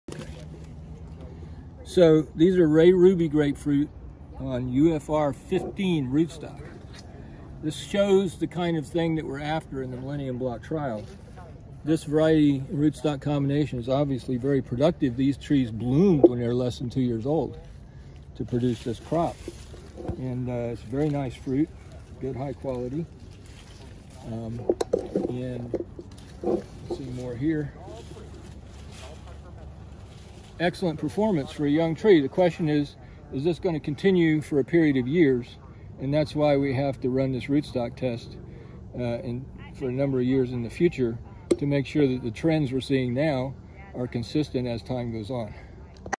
Pete Spyke presents a high-quality citrus rootstock
Longtime Indian River District citrus grower Pete Spyke presents a new scion rootstock combination that tolerates citrus greening and produces high quality fruit